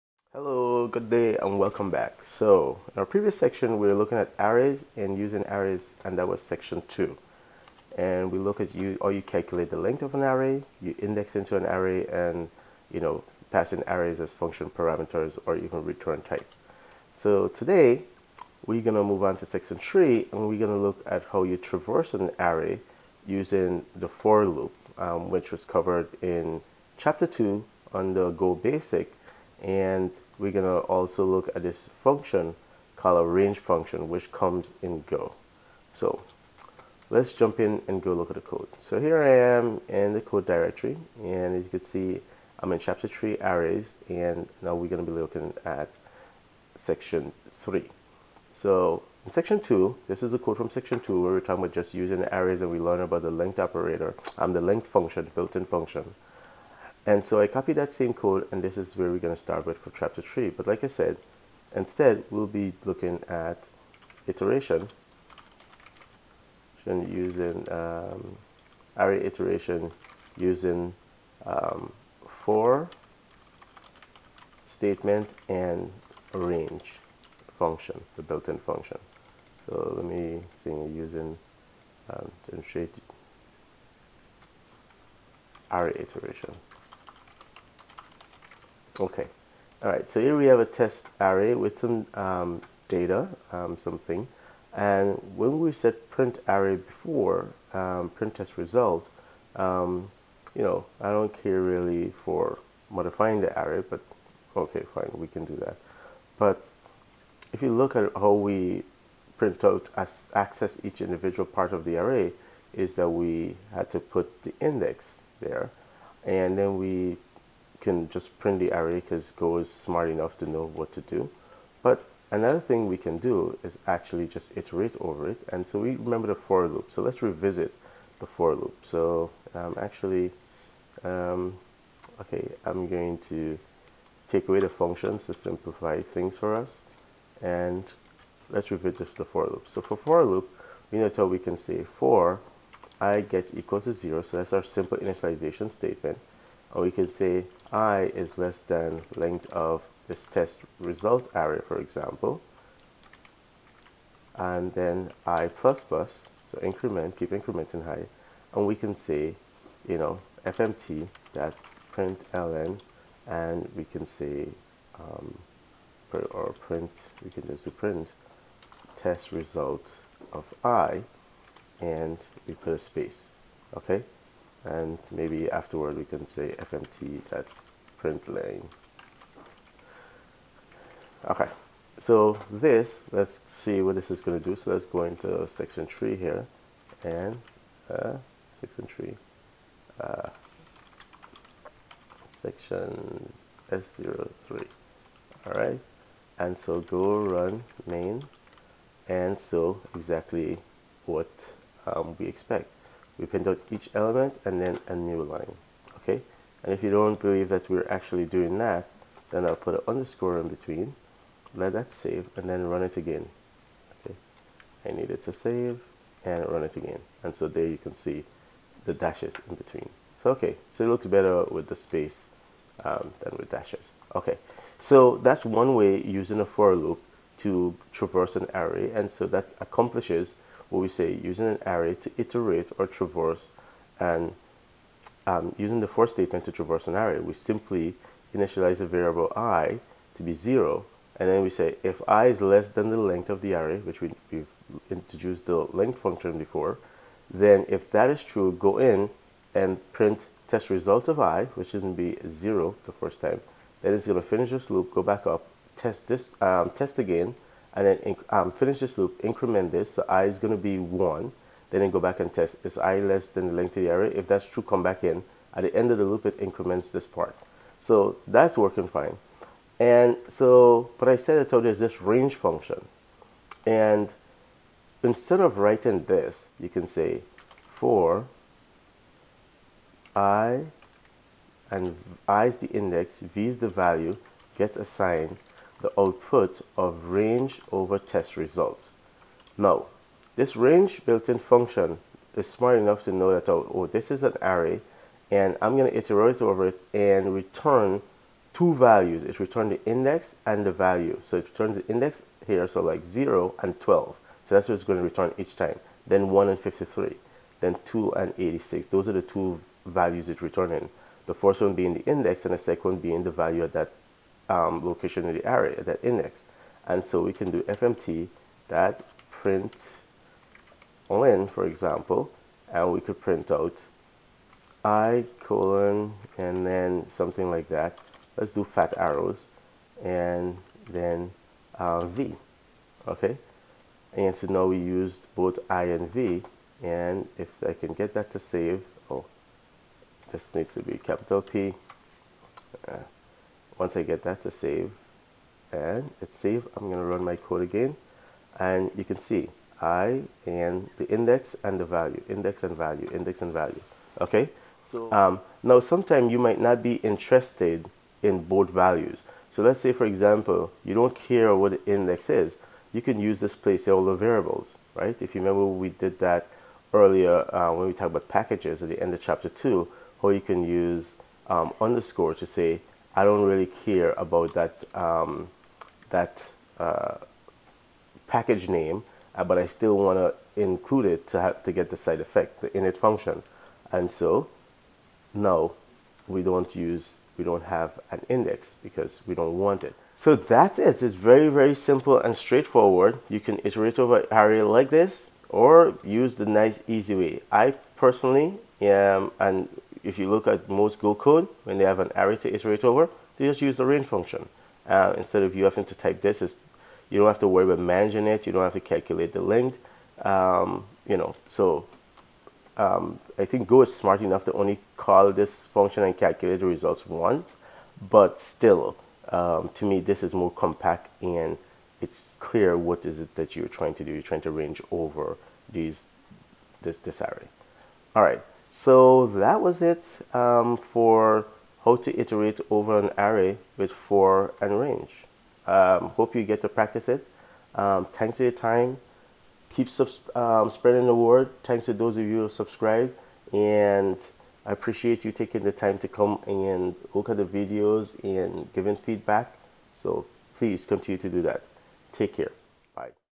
03.03 - Iteration Using 'for' and range in Go
Iterating over the elements of an Array using 'for' loop and range() function.